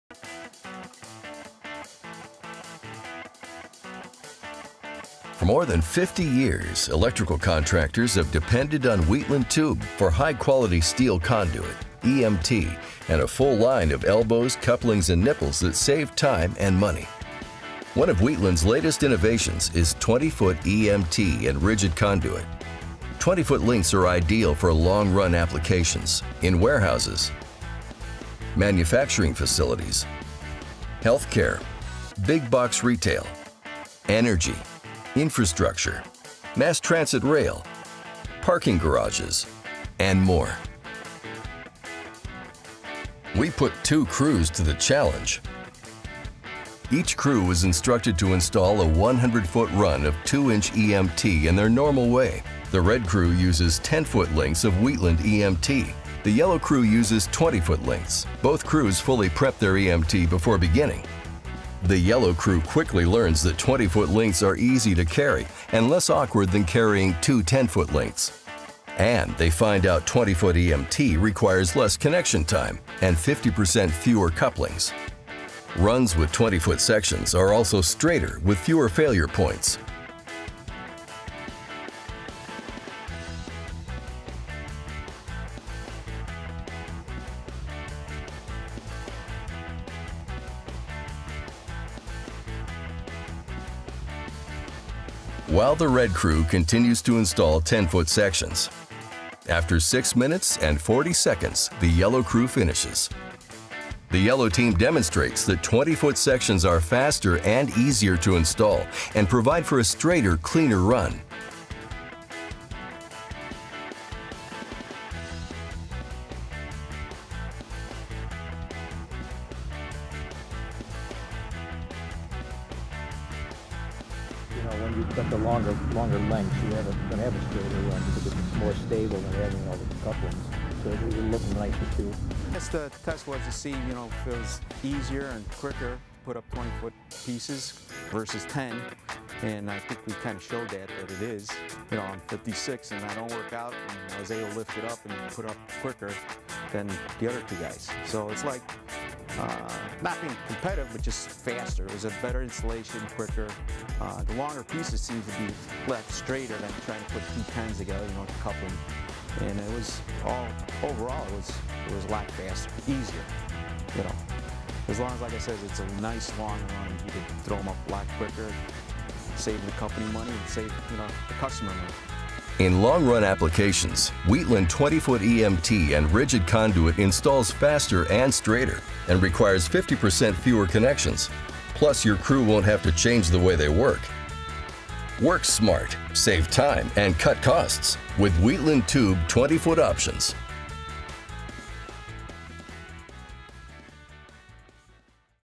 Wheatland Tube 20' EMT & Rigid Conduit Video | Zekelman Industries
One of Wheatland's latest innovations is 20-foot EMT and rigid conduit. 20-foot lengths are ideal for long run applications, warehouses, manufacturing facilities, healthcare, big box retail, energy, Infrastructure, mass transit rail, parking garages, and more.

In this video, we put two crews to the challenge. Each crew was instructed to install a 100-feet run of 2-inch EMT in their normal way. The red crew uses 10-foot lengths of Wheatland EMT. The Yellow crew uses 20-foot lengths.